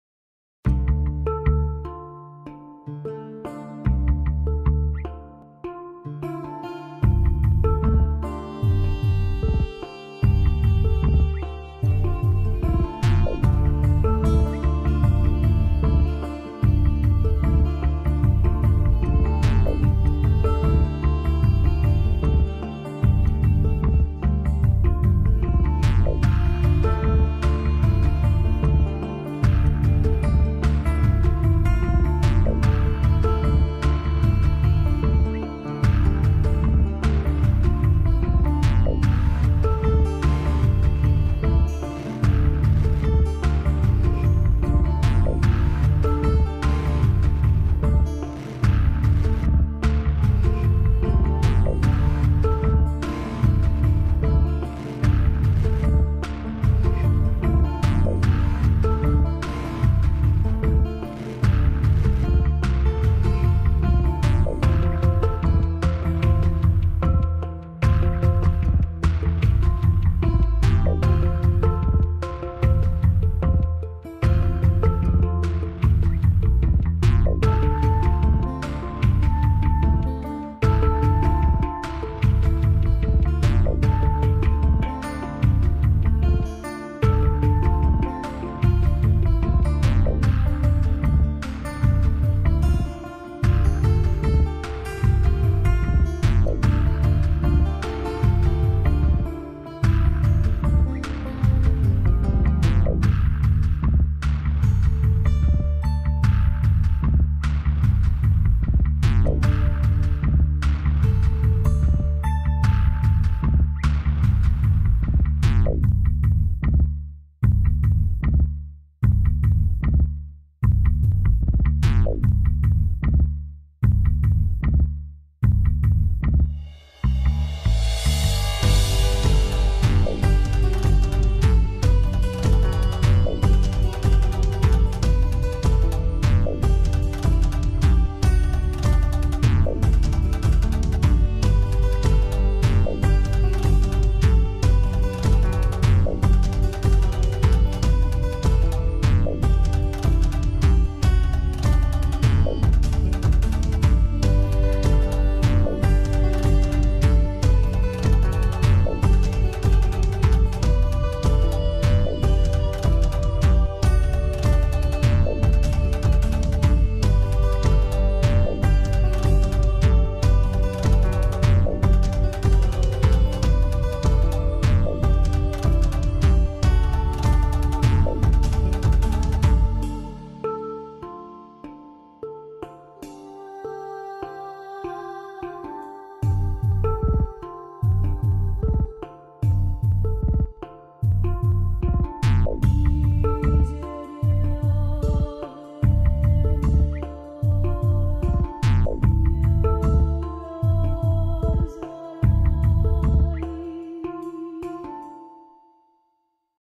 Speedpainting: Winged Cracked Ying Yang
This is what I want tattoo on me. Hope you enjoy a rendition of the Ying Yang symbol.